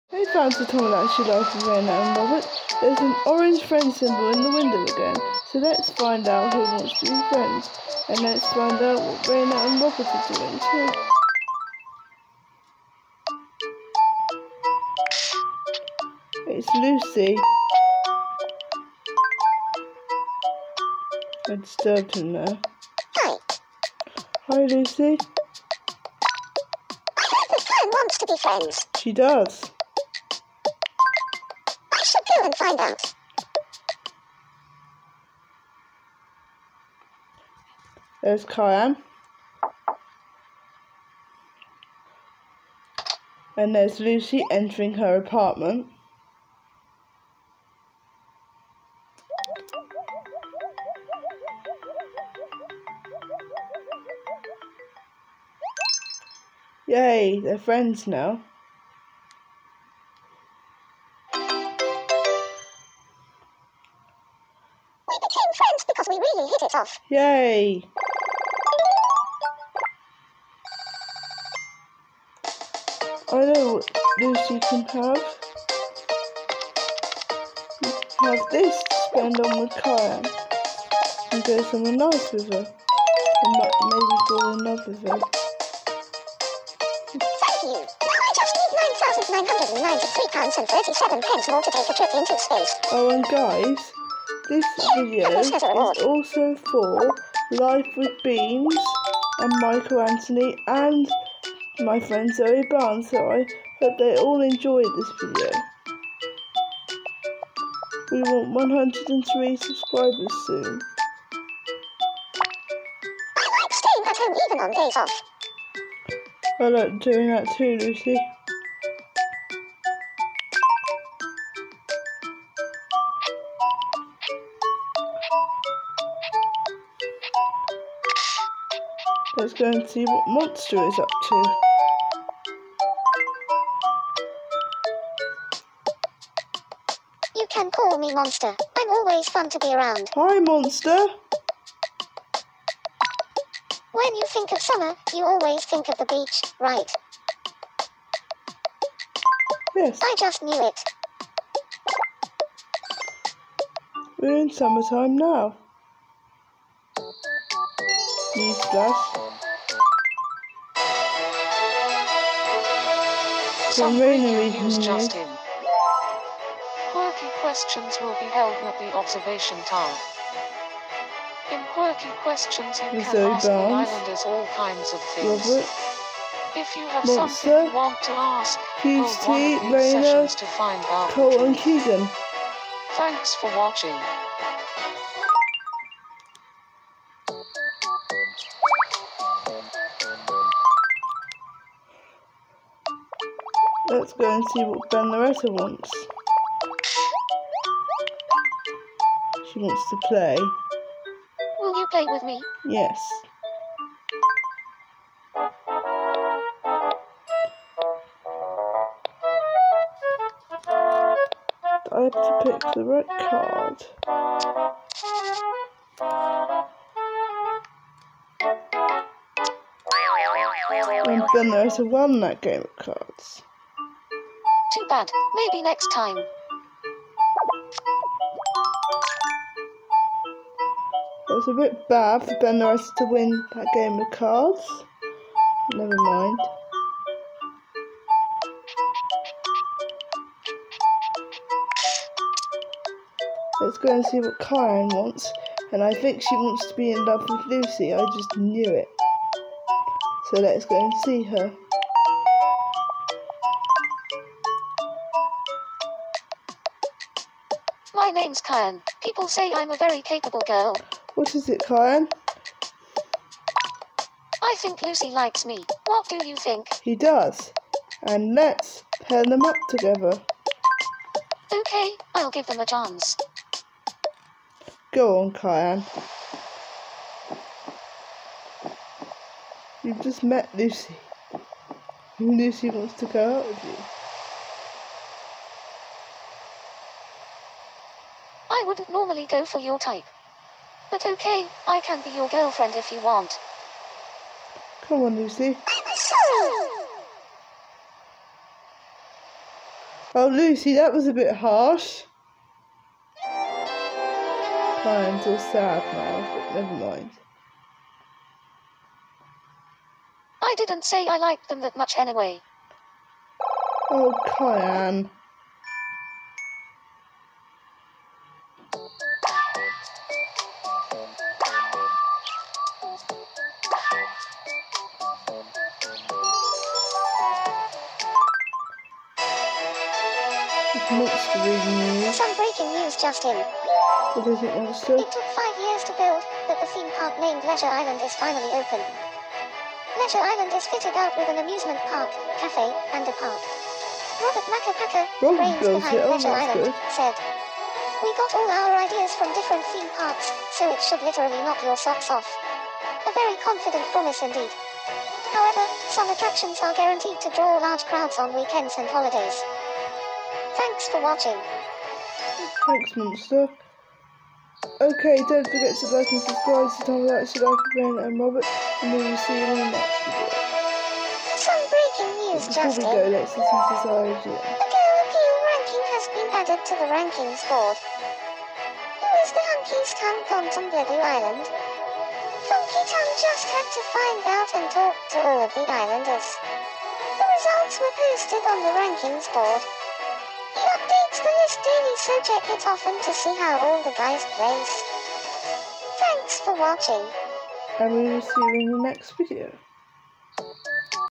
LUCY Meets Kyan
In this episode LUCY Meets Kyan and she is very pleased to see him But 2 hours later Kyan thinks that LUCY likes her and then meets him on the beach and asks LUCY to be her boyfriend He says no